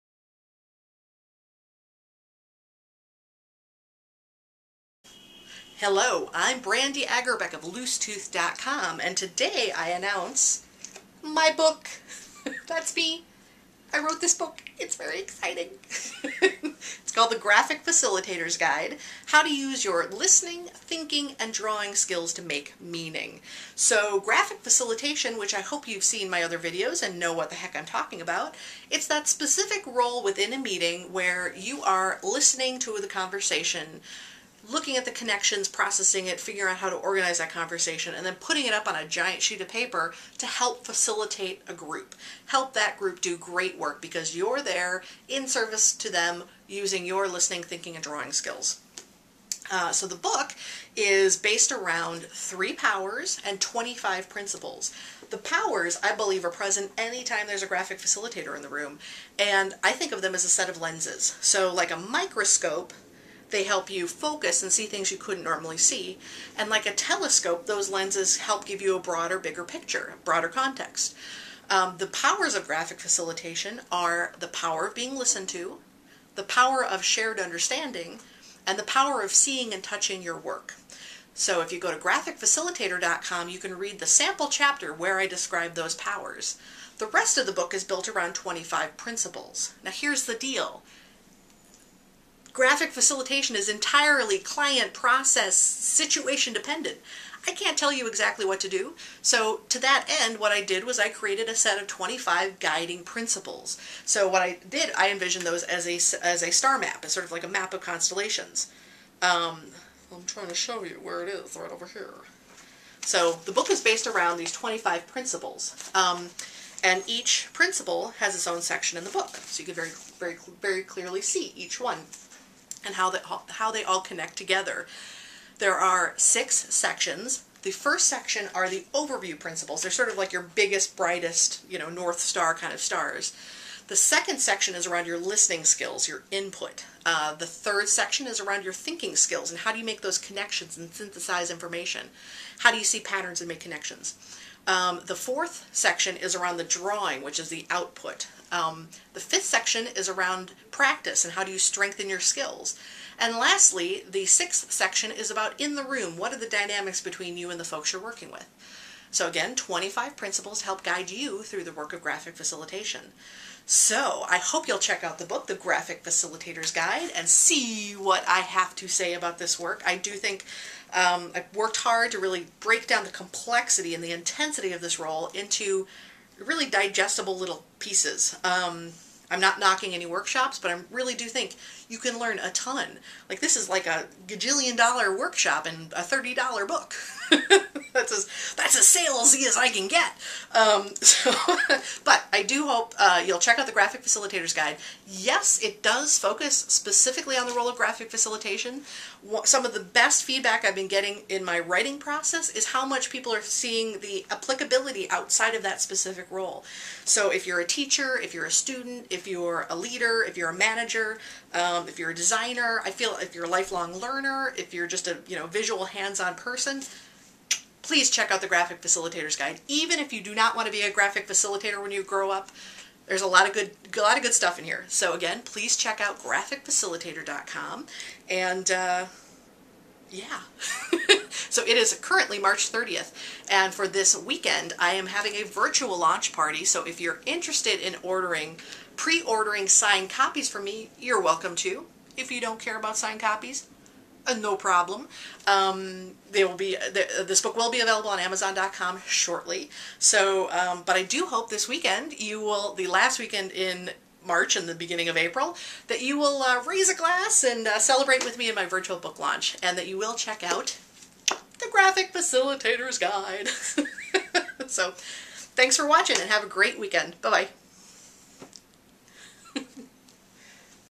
Brandygram 03.31.12: Announcing The Graphic Facilitator's Guide by Brandy Agerbeck
Virtual book launch party! to celebrate my new book: The Graphic Facilitator's Guide: how to make meaning with your listening, thinking and drawing skills.